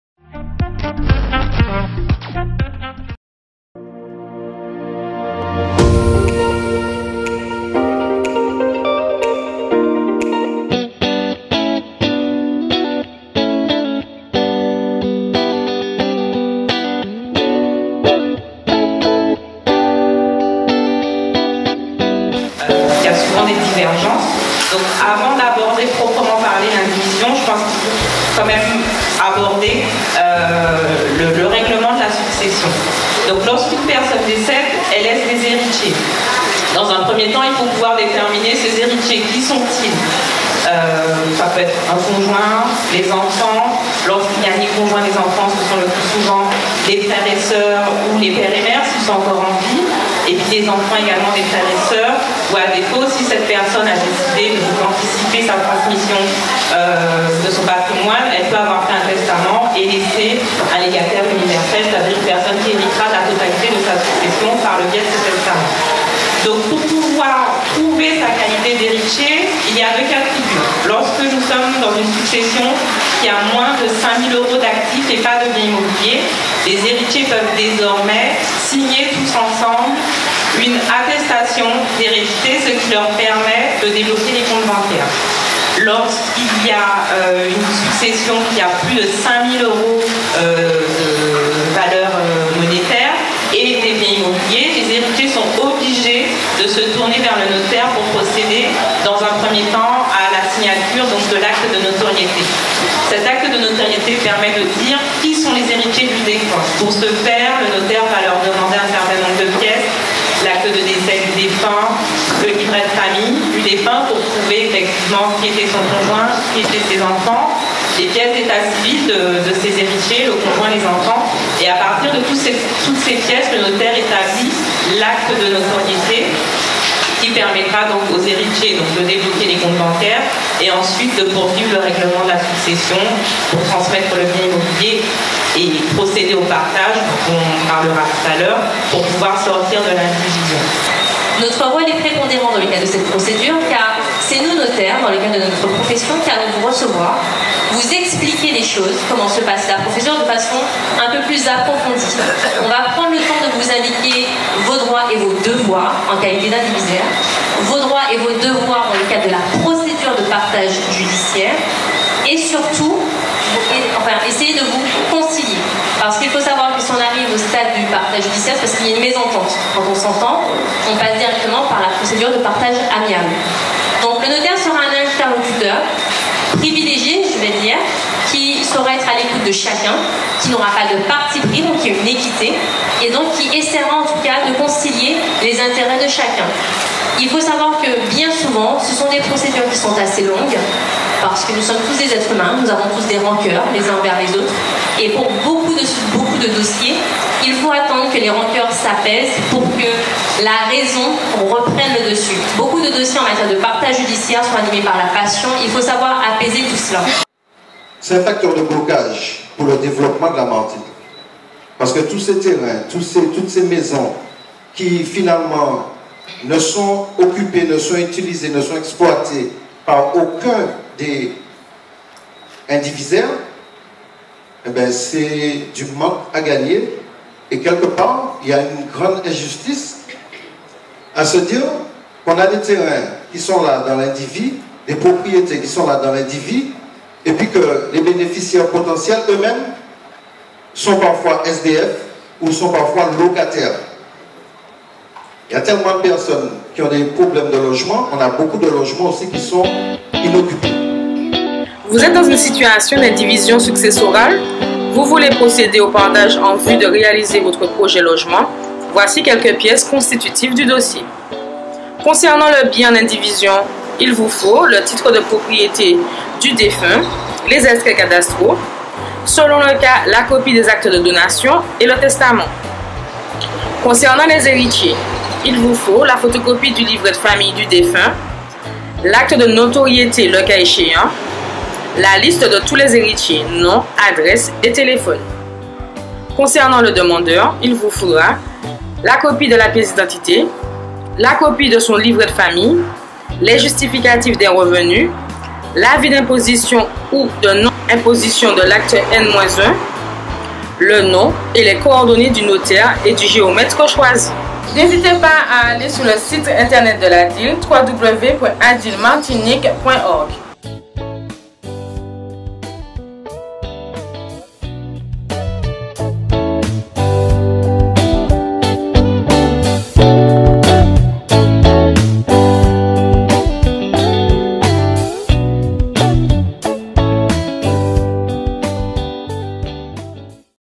Sortir de l’indivision successorale : Pourquoi, Comment et Quelles aides?
▶RUBRIQUE : VILLE DE DUCOS

Conférence d’information sur le thème

Dans le cadre de la poursuite des actions de sensibilisation du public sur la nécessité de sortir de l’indivision, la Communauté d’Agglomération de l’Espace Sud et le CAF Martinique ont organisé en partenariat avec l’ADIL (Agence d'information sur le logement de la Martinique) Martinique et la Chambre des Notaires une rencontre qui avait également pour objectif d’informer les administrés du Sud de l’existence de dispositifs financiers capables de leur apporter une aide pour le règlement de ces dossiers souvent onéreux.

Cette conférence animée par l'élue Marcelle RENARD Adjointe Chargée de l'environnement, le Service Urbanisme de la Ville, les notaires M.Julien MARRY, Mmes Audrey BORDES et Alorie ARNAUD, Mme BAURAS Présidente de l'ADIL, Mme Béatrice PALMYRE Conseillère Juriste chargée de mission indivision de l'ADIL , Mme BARRAT Marlène Cheffe du service des politiques urbaines de l'habitat, Mme Stéphanie NORCA représentant la CTM (Collectivité Territoriale de Martinique) et M. Henry PAQUET Vice-Président de l'Espace Sud s'est déroulée le vendredi 05 janvier 2018 à 18h au Restaurant scolaire du bourg de Ducos